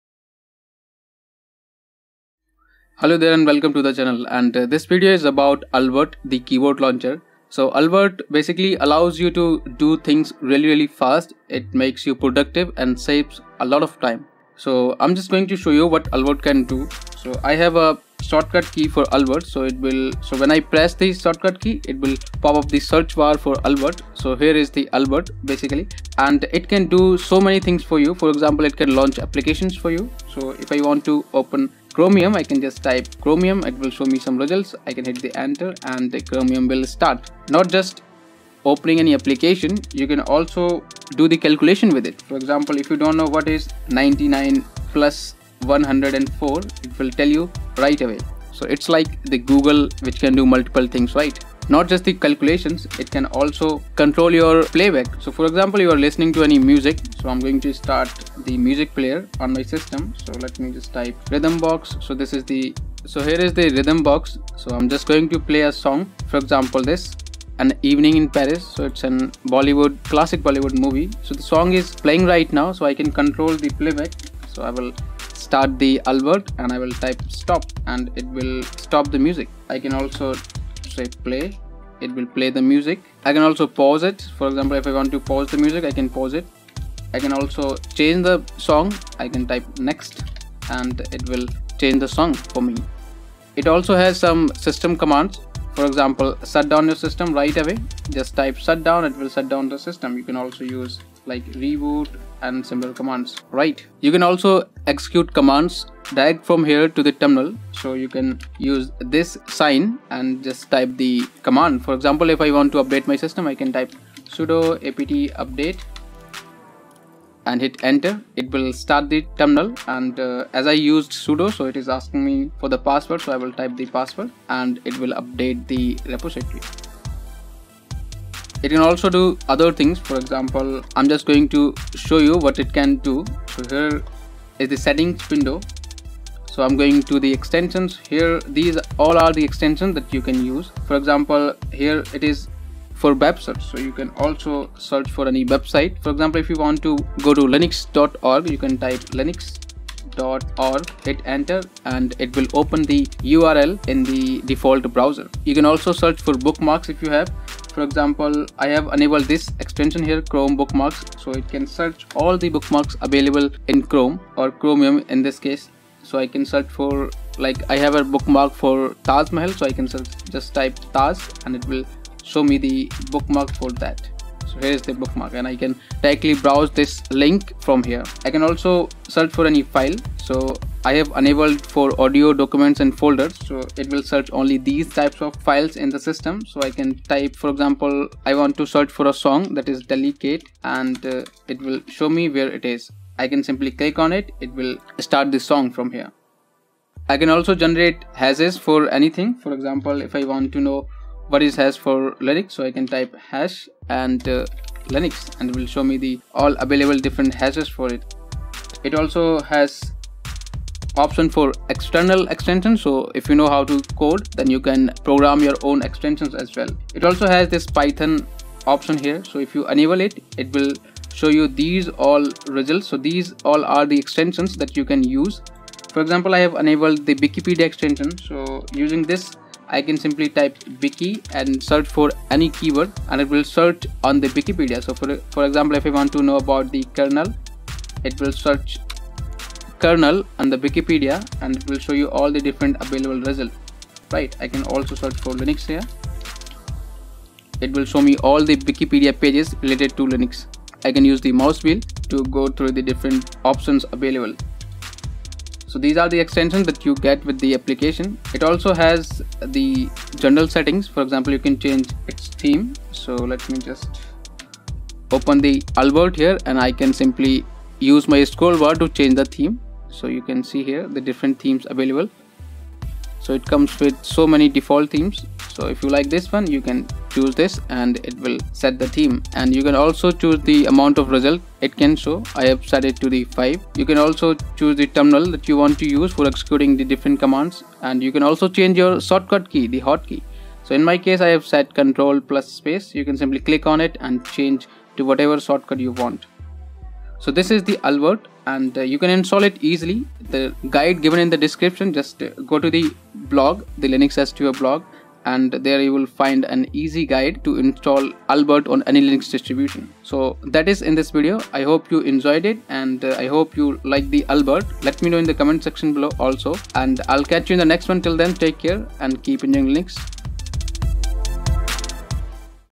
Albert - An amazing application launcher for Linux (Ubuntu, Mint, Kali LInux, Manjaro)
Learn about Albert - An amazing keyboard launcher for Linux. It's an application that allows you to do things from the keyboard which you normally have to carry out with a mouse. Having one makes your interaction with your computer very easy and quick.

If you stuck with anything don't forget to ask in the comment section.